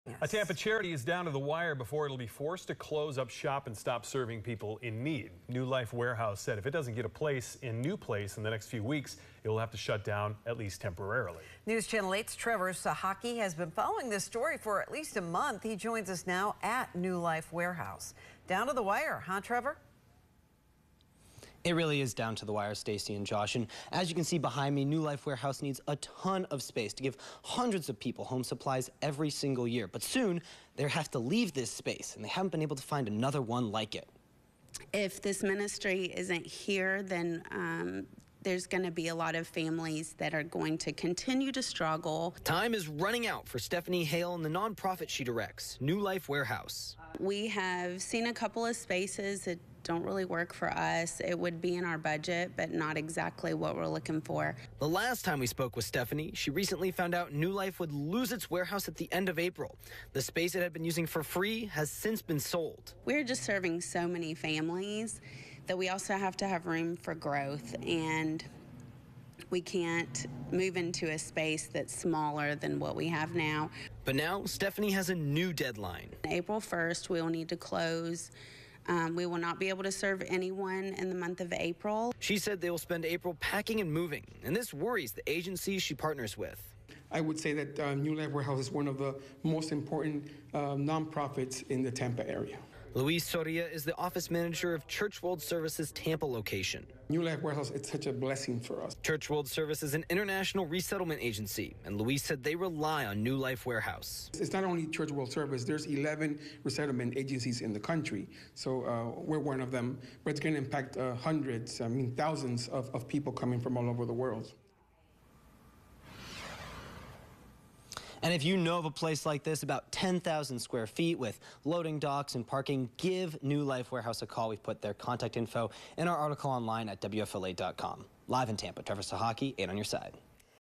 Tampa nonprofit closing its doors in less than a month with nowhere to go
After finding out earlier this year that New Life Warehouse would lose its space at the end of April, Haile recently announced that the nonprofit would be shutting its doors at the end of March and not serving any more clients to pack up and move out.